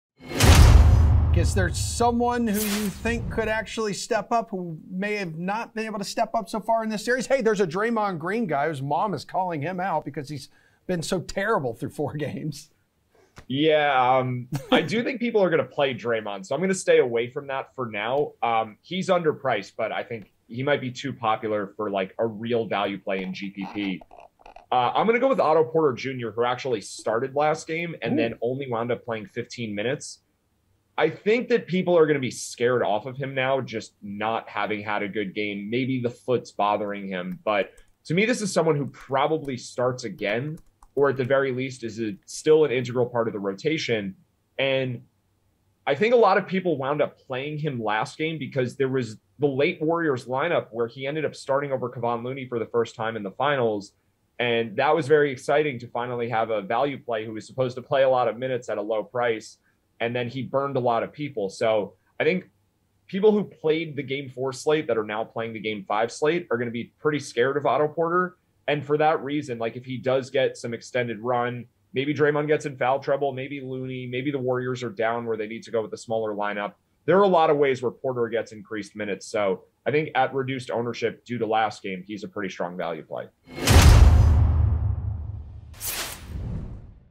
How Much Value Can You Get Out of Otto Porter Jr. Tonight?
DraftKings contributor Matt Meiselman joins The Sweat to give a DFS and Sportsbook preview of tonight’s Game 5 action between the Boston Celtics and Golden State Warriors.